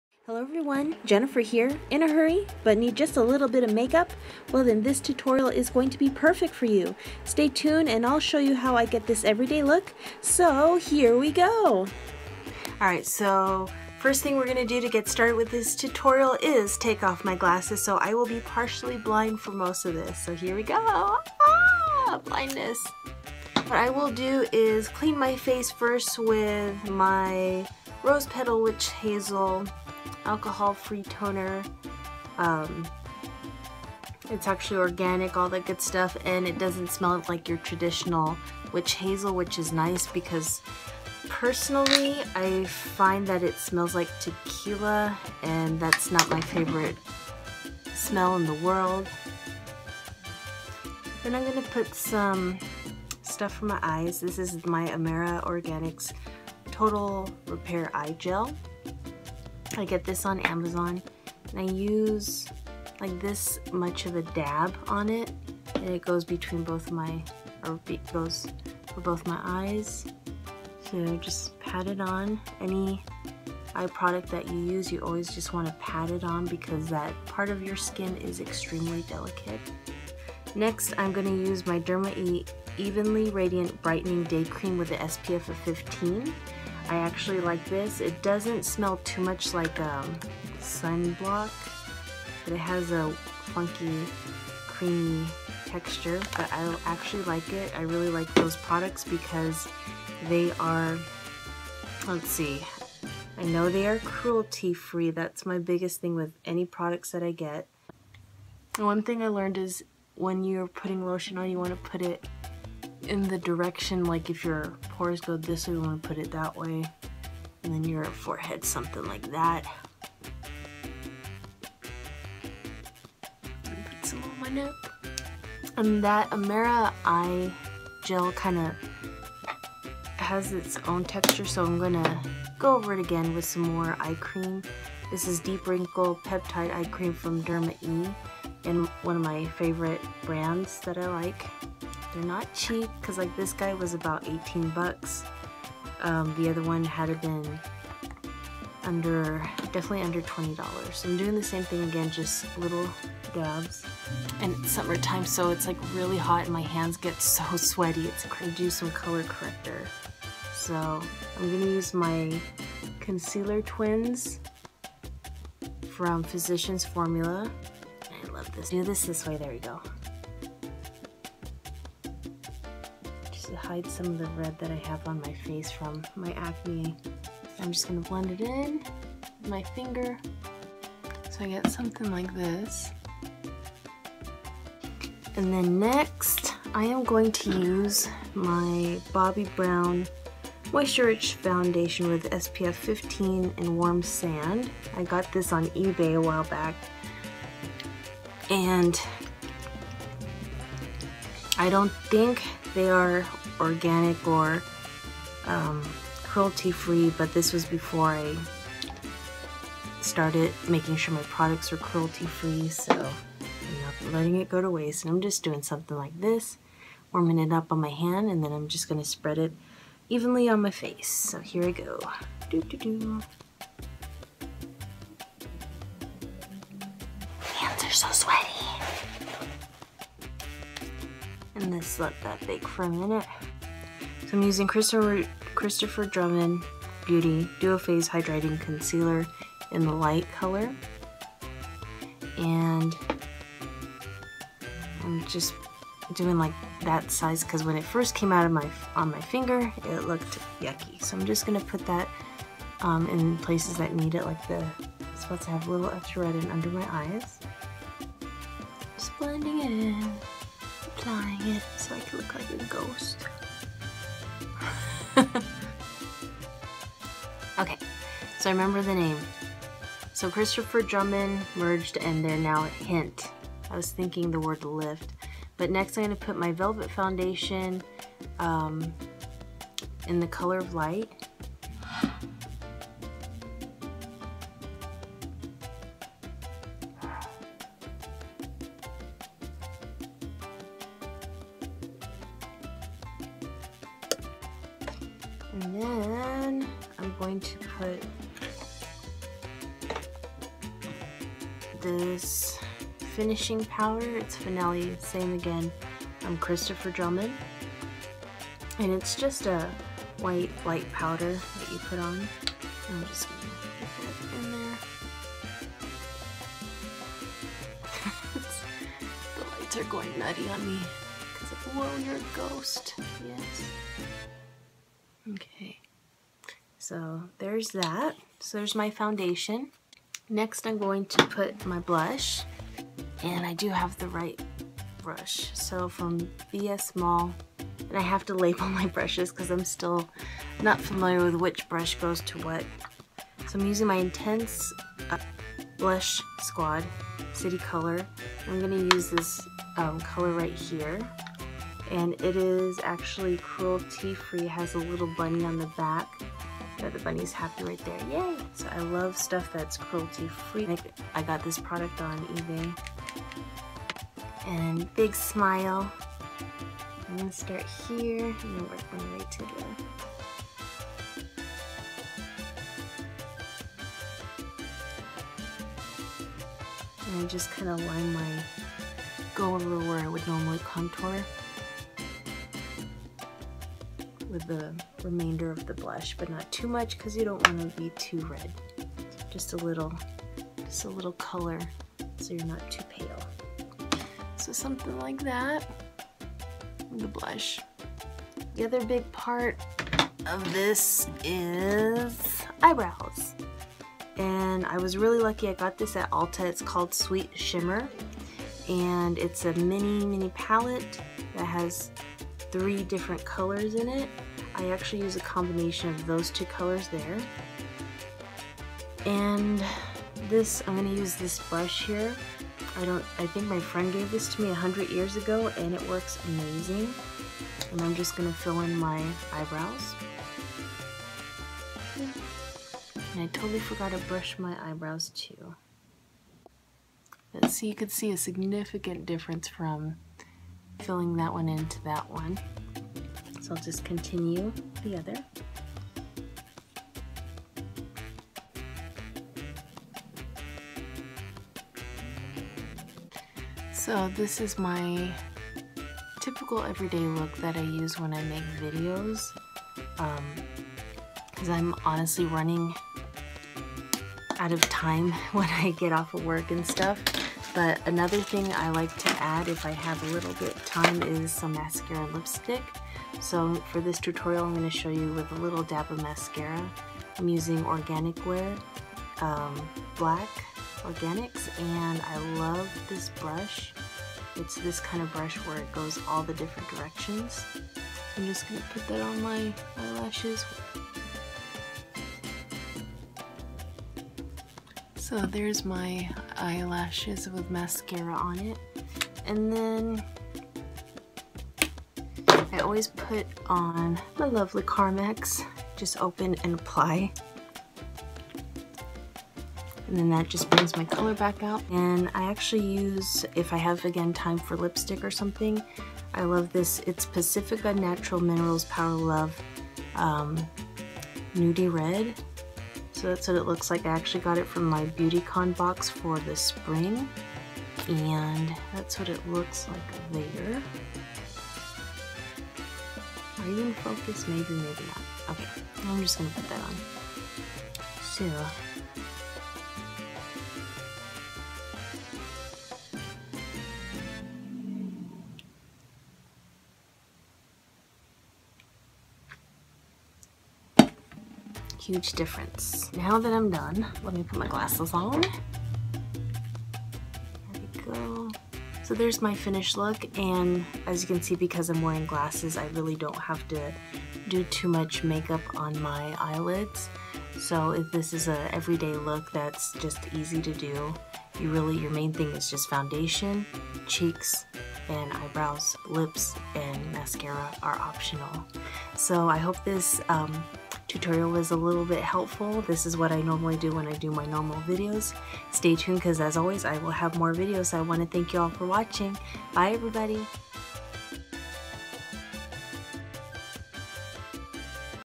My everyday make up look TUTORIAL
Super simple every day make up tutorial. It's great when you're in a hurry. 3 main elements are foundation, blush and eyebrows. Sorry for my streaky make up in some points in the video it was crazy hot that day. Otherwise watch and enjoy!

Created and edited with pocketvideo.